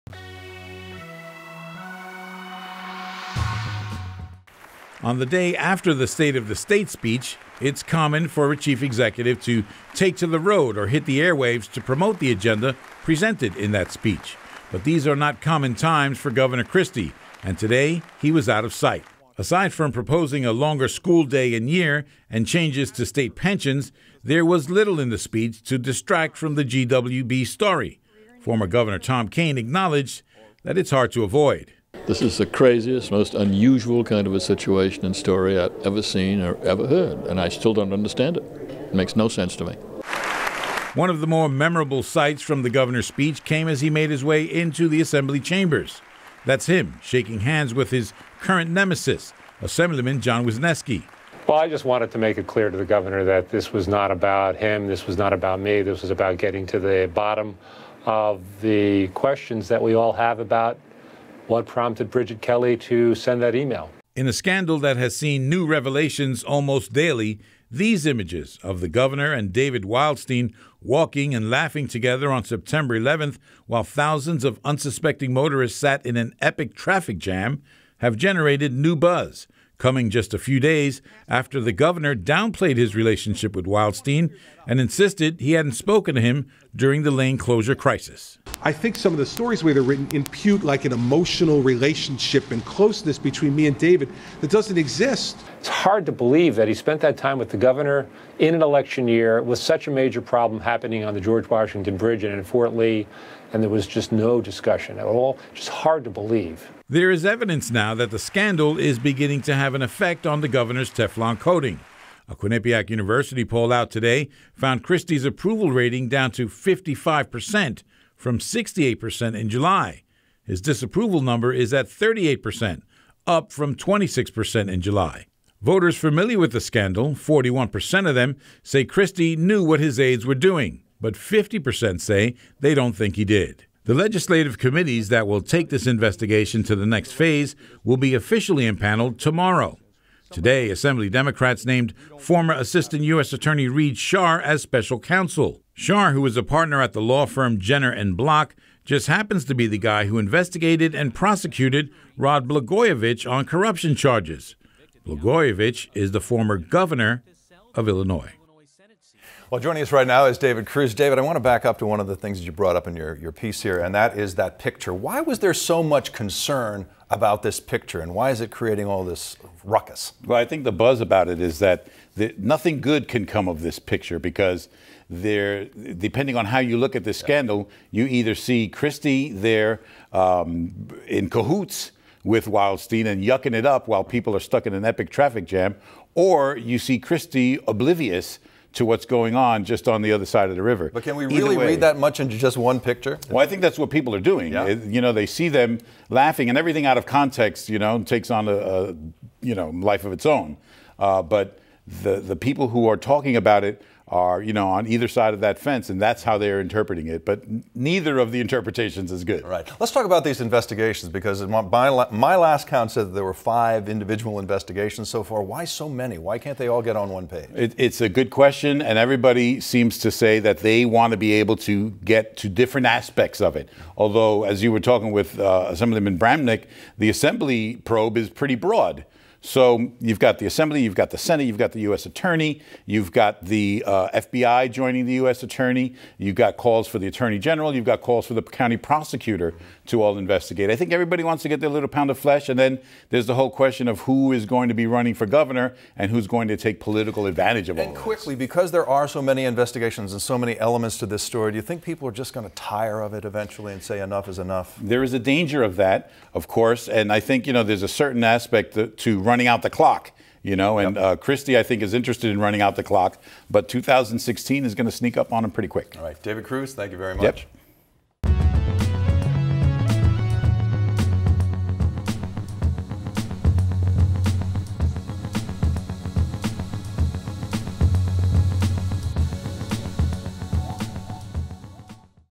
GWB Controversy Continues as New Photos of Christie, Wildstein Surface
Images of Gov. Christie and David Wildstein have surfaced showing the pair together Sept. 11, amid the controversial GWB lane closures. For more New Jersey news, visit NJTV News